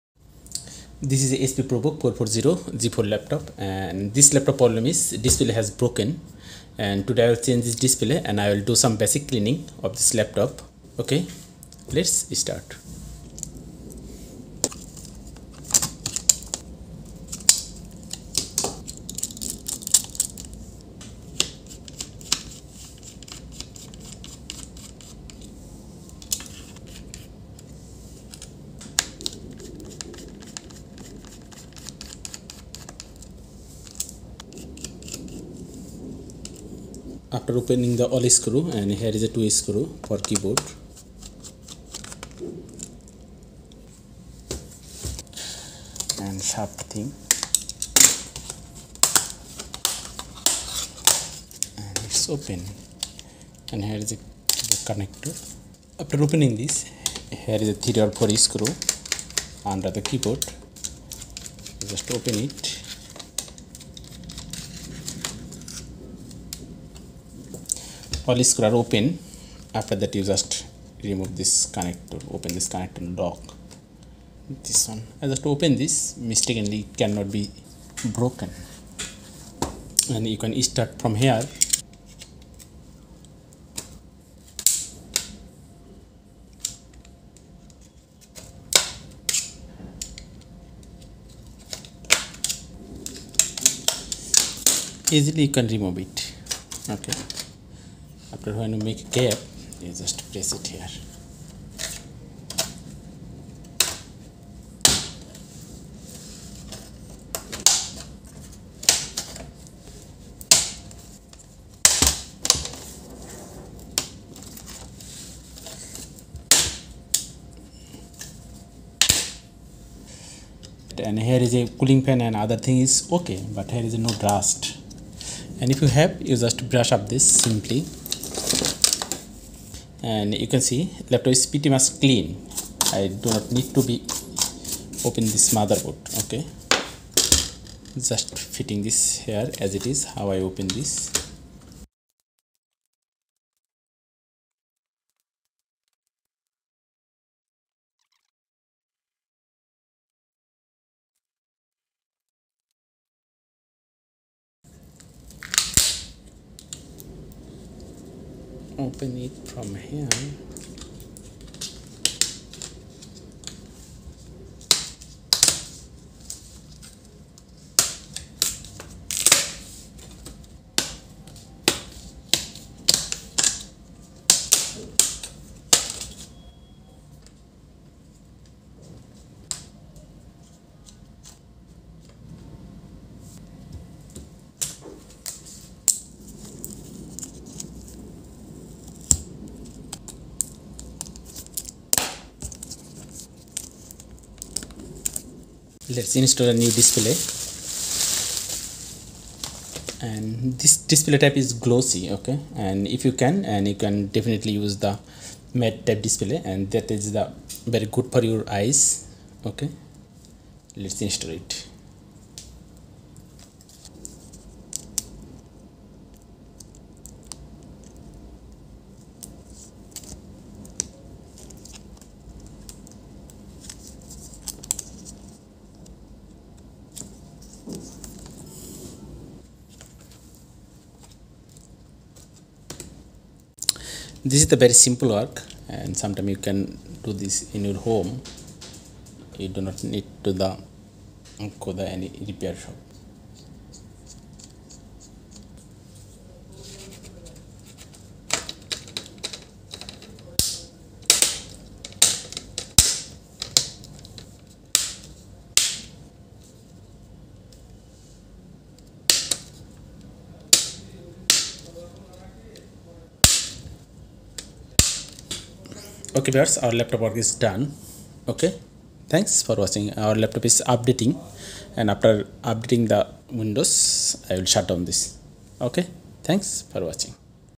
Hp Probook 440 g4 restoration and display changed basic cleaning.
Dear friends in this video i will show you how to change display hp probook 440 g4 laptop